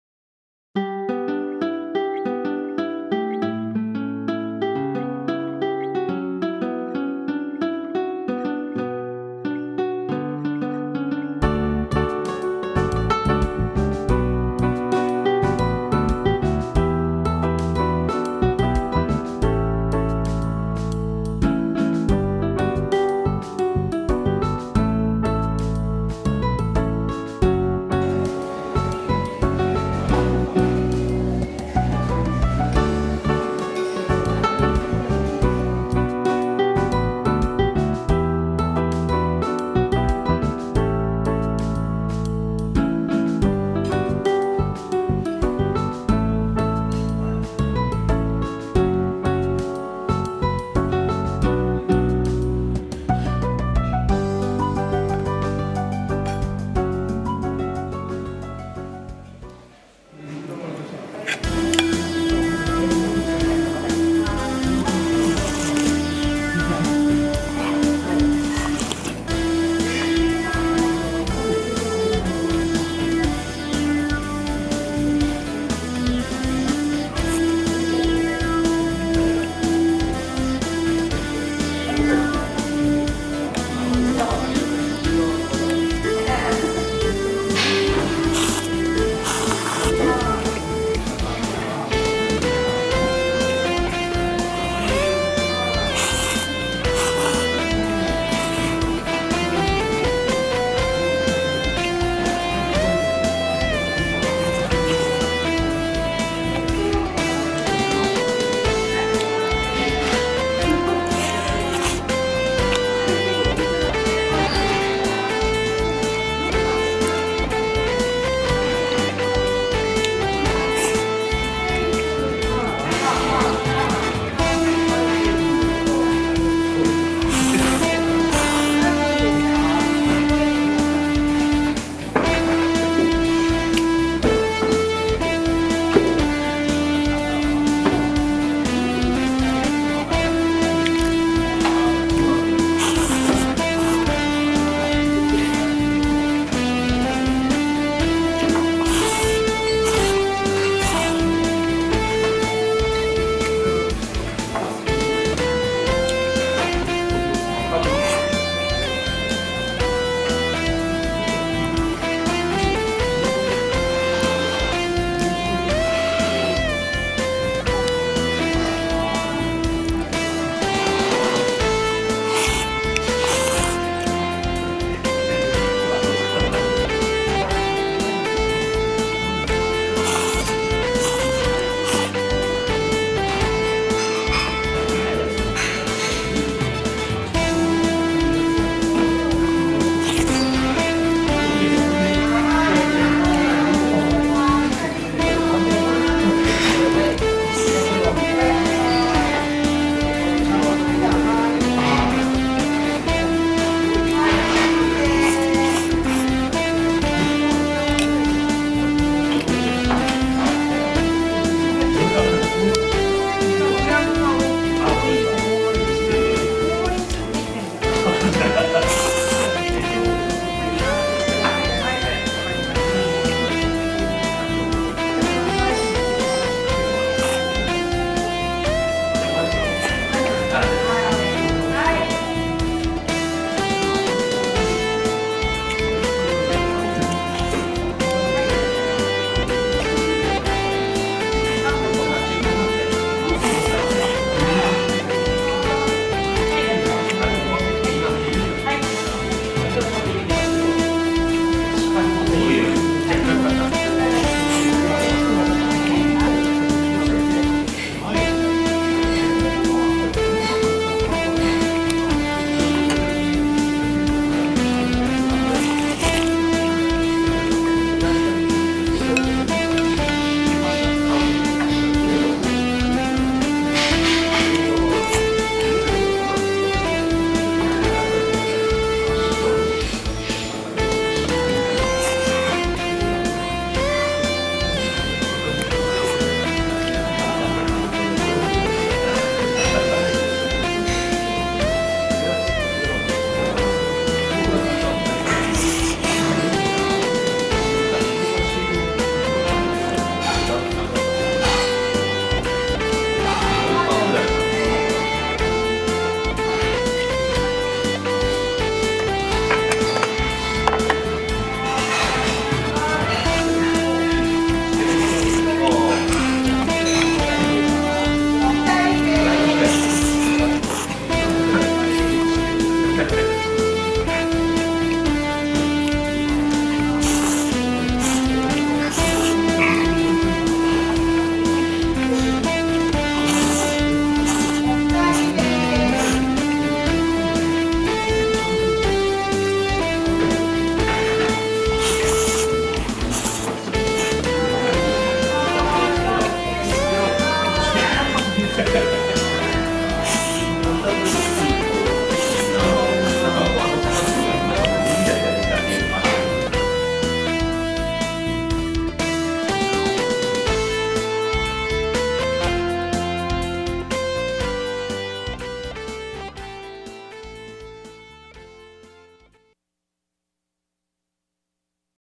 【麺や すみよし】 これって竹岡式ですか？ あぶりチャーシューメン ＃668
安房郡鋸南町台風15号の大きな被害を受けた地域で昼食に初めて入ったお店が麺や　すみよしさんです　竹岡式との噂があり　検証してみます